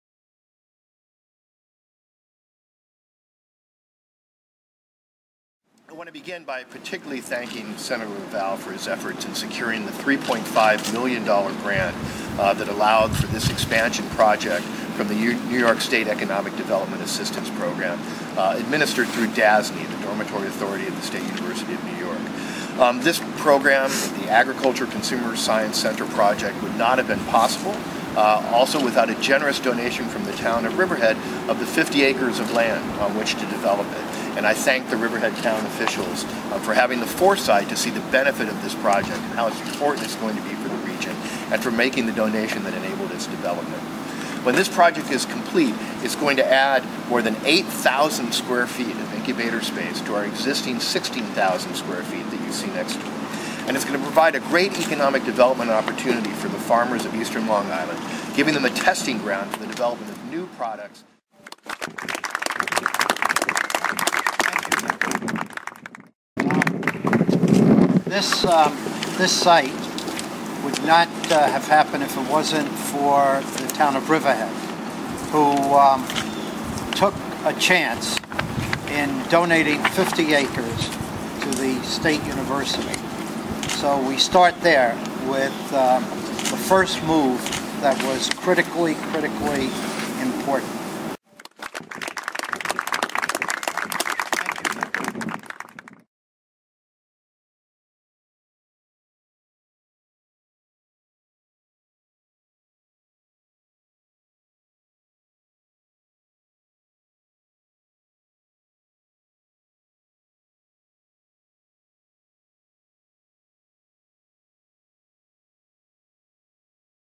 AgriCenter Ground Breaking Oct. 24
Nearly one year after announcing the open bidding process for the construction of a new Agriculture Consumer Science Center (ACSC) at the Stony Brook University Calverton Business Incubator, Senator Kenneth P. LaValle and other local dignitaries celebrate the groundbreaking and construction advancement of the planned Center. The project expected completion date is summer 2012.

The ACSC is being developed to help regional wine and agricultural producers and local entrepreneurs develop new, marketable consumer items and enhance their product lines through access to state-of-the-art shared processing areas and collaborations with researchers, scientists, and professionals at Stony Brook University, Brookhaven National Laboratory, and the NYS Small Business Development Center.

In March 2010, Stony Brook announced that through the efforts of New York State Senator Kenneth P. LaValle, the University received a $3.5 million grant from the New York State Economic Development Assistance Program (NYS EDAP) for the construction of the ACSC at the Calverton Business Incubator, which was established by SBU in 2005 with a focus on the development of aquaculture, agriculture and environmental technologies. Funding is being administered by the Dormitory Authority of the State of New York (DASNY).